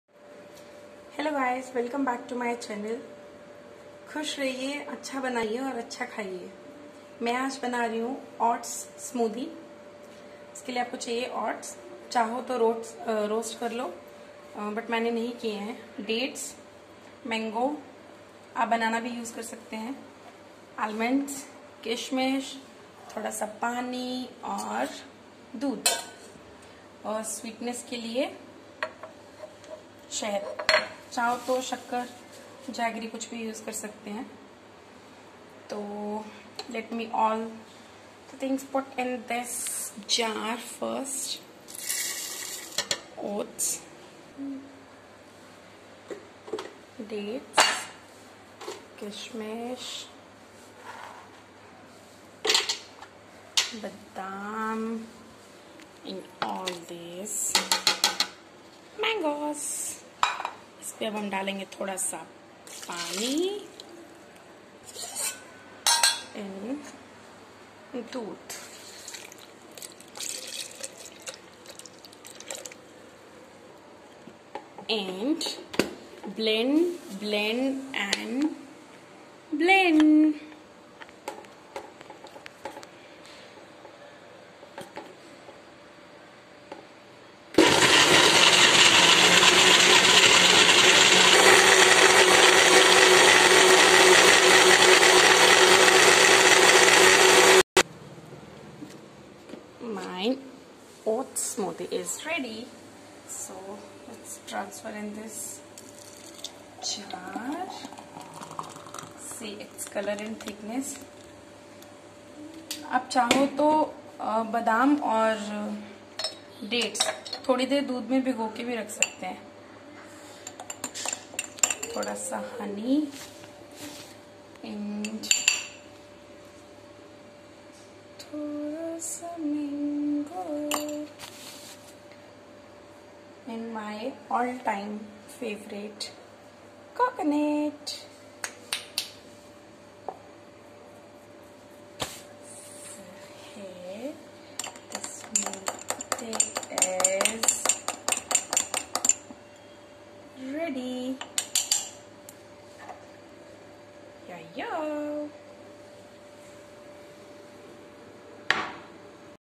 oats smoothie | weight loss smoothie | Weight loss healthy smoothie
Ingredients. almonds + kismish + mango + milk + water + honey blend blend and blend enjoy healthy and yummy oats smoothie.
. Benefits of oats

Provides antioxidants.

Promotes healthy bacteria in your gut.

Helps you to feel full to manage your weight.

Eases constipation.

Relieves skin itching and irritation.

Lowers your chance of colon cancer.